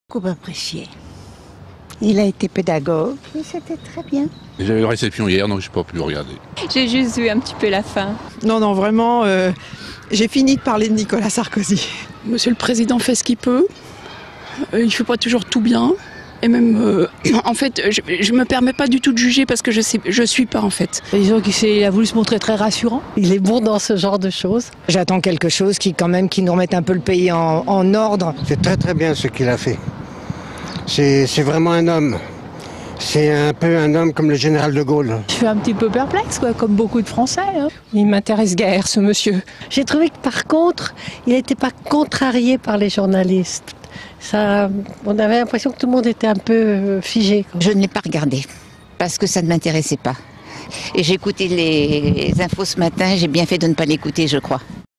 Intervention de Sarkozy sur TF1: Vos réactions (Essonne)
Vous voulez conserver ce reportage ?
 Les essonniens réagissent au lendemain de l'intervention du chef de l'état sur TF1
. (DROITS RESERVES . Pour toute exploitation sur TV, web, mobile, aller